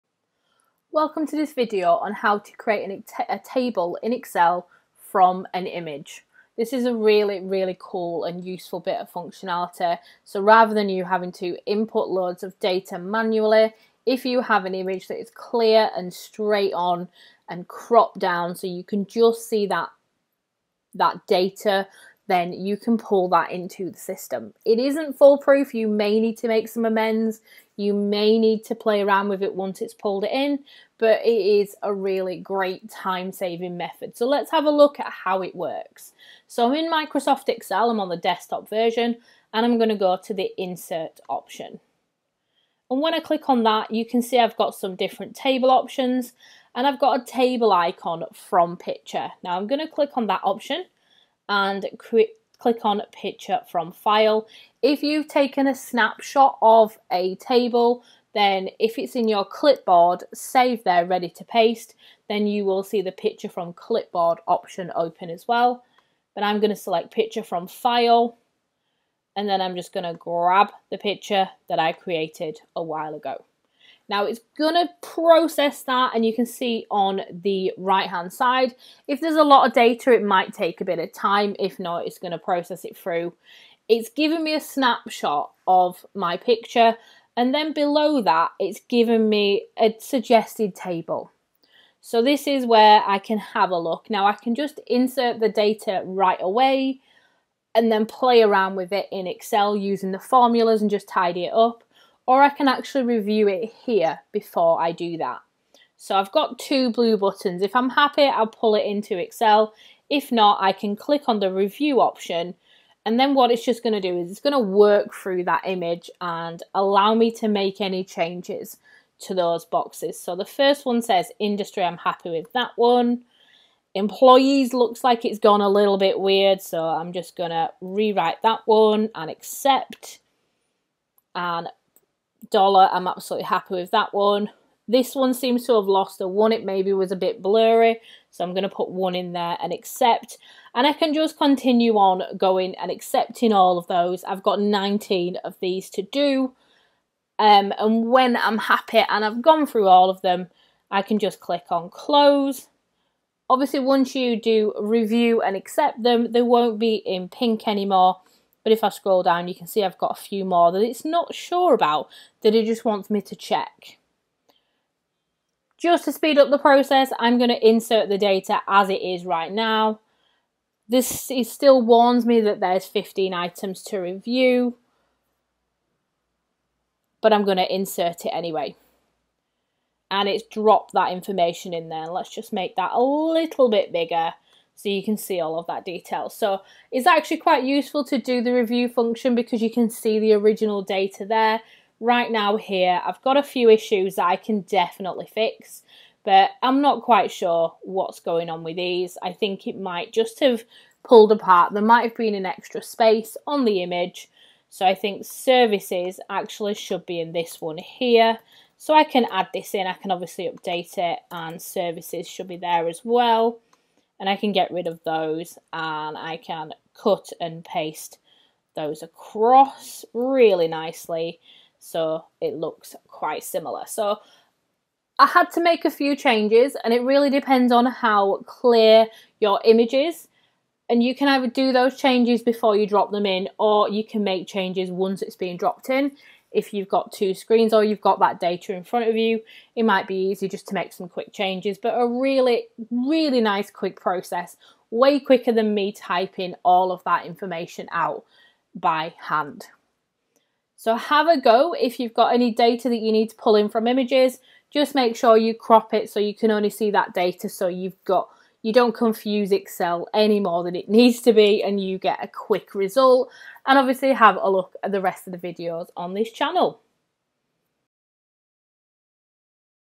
How to | Microsoft Excel | Create a table from an image with AI
This video will look at how to create a table from an image on the Mac version of Microsoft Excel using AI. This function means you don't have to input data manually, remember you may need to review and make some changes before the table is created. You can also make changes after the creation of the table.